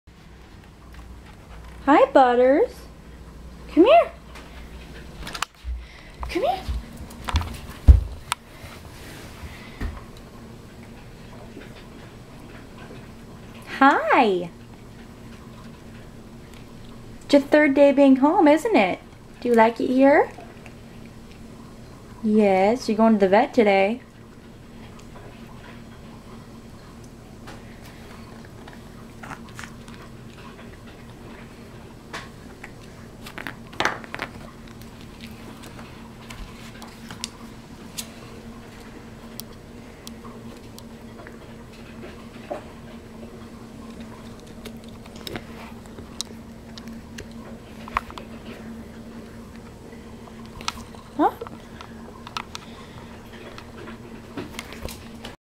Adorable Boston Terrier Puppy
My baby Boston Terrier named Butters.